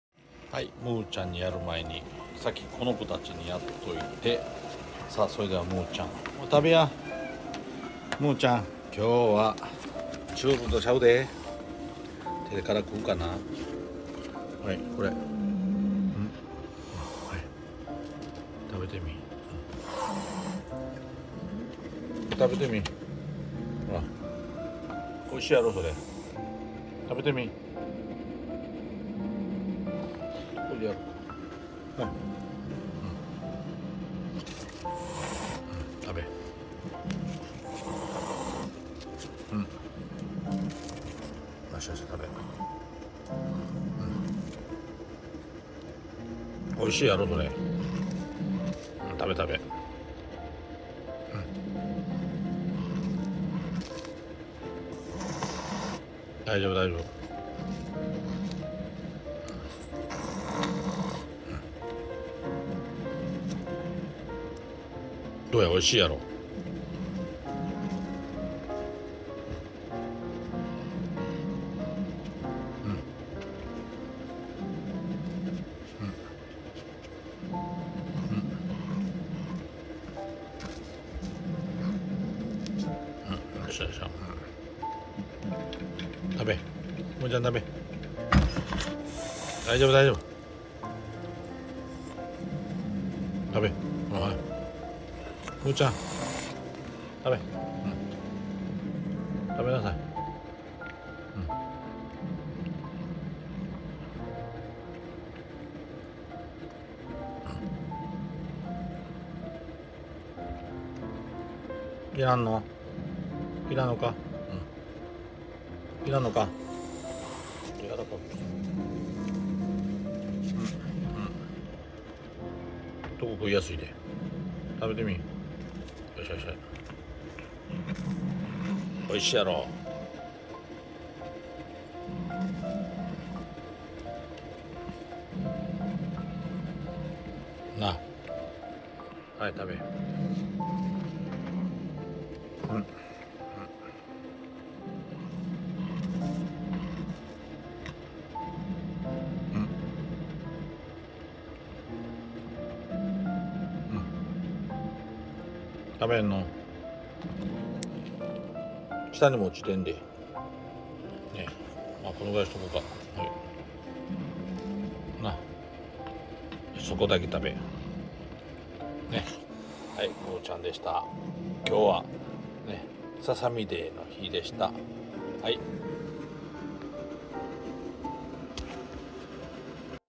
ムーちゃんに素手でドライササミを与えてみたら
365日の訓練を終え 現実の様子は？これだ！

[里親募集]

2ヶ月を過ぎた子猫から譲渡させて頂きます

非通知不可